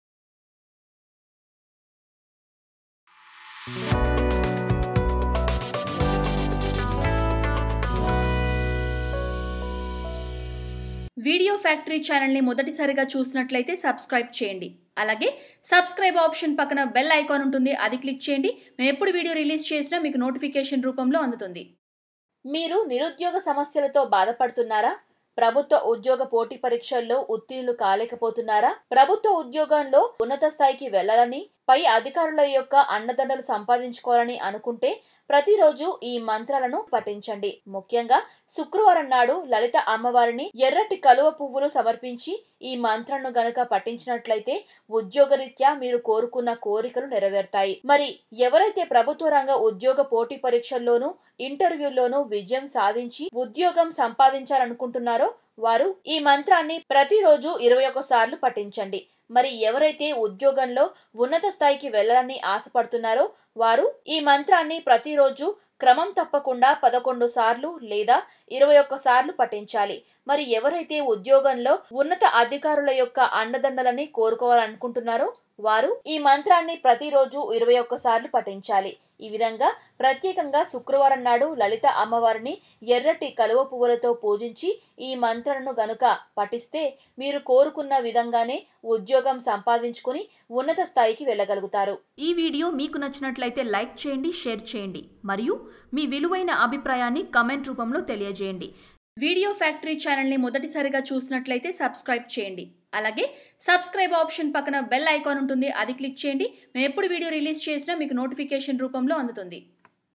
100% Government Job రావాలంటే మీరు జపించాల్సిన చిన్న మంత్రం|Mantra For Govt Job|Video Factory
100% Government Job గవర్నమెంట్ జాబ్ (ప్రభుత్వ ఉద్యోగం) రావాలంటే మీరు జపించాల్సిన చిన్న మంత్రం.
Mantra For Government Job and govt job mantras to get the job very fast for sure.